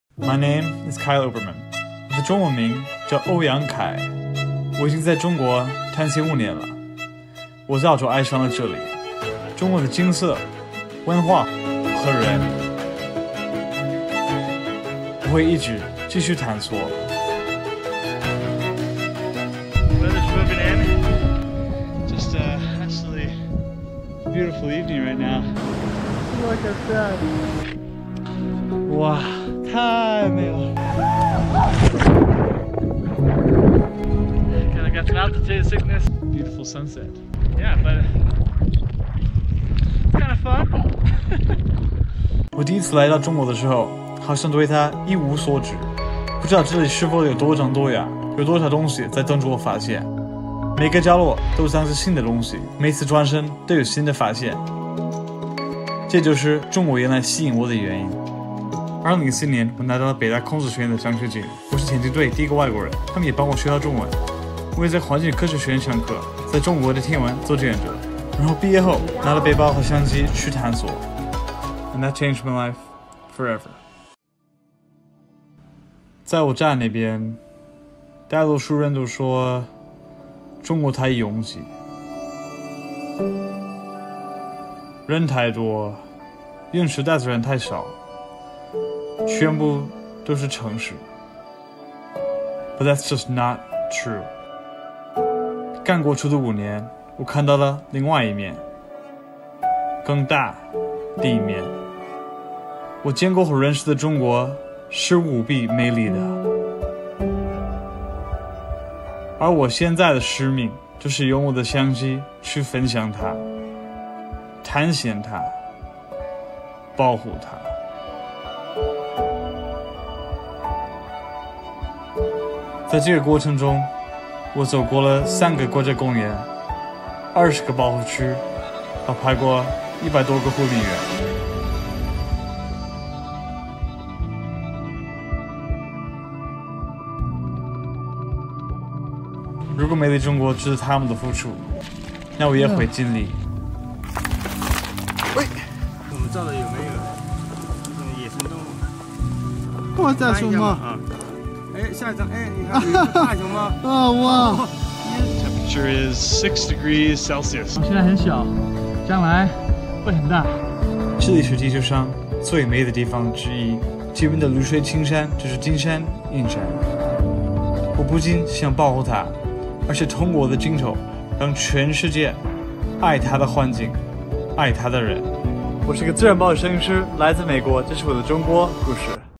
美国小伙眼中的大美中国China and I
一个美国小伙探险中国的大美河山。
An American makes adventures to great rivers and mountains of China.